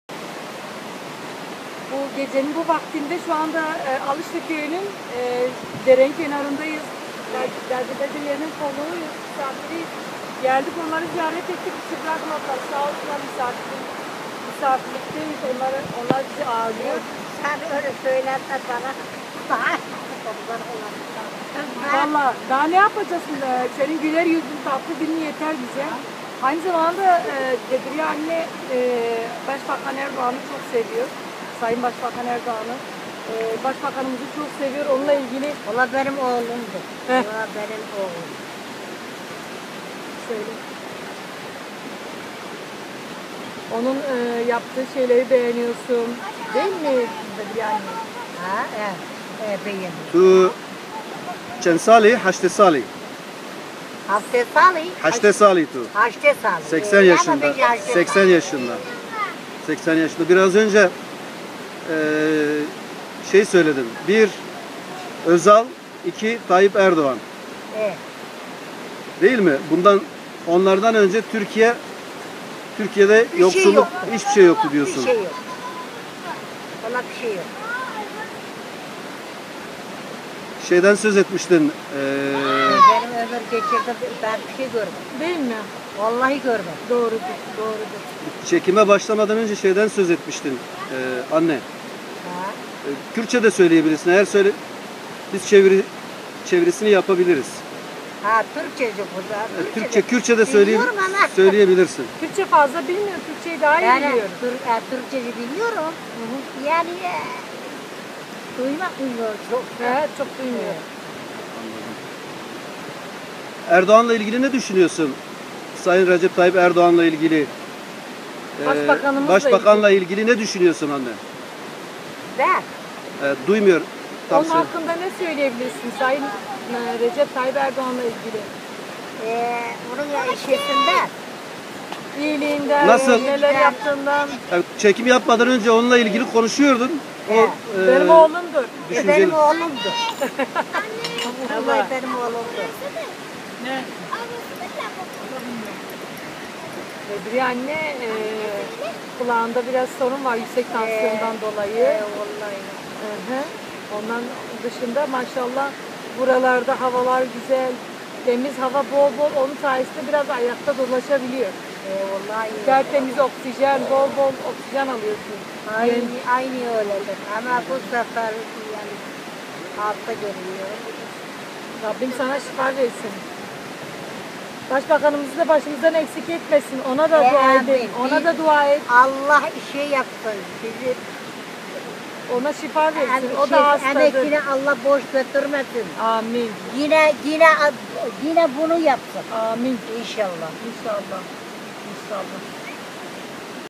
Bedriye ninenin Başbakan Erdoğan sevgisi . ''Onu Oğlum gibi seviyorum ''
Bedriye nine ''Başbakan Erdoğan Benim oğlumdur. Onu Oğlum gibi seviyorum ''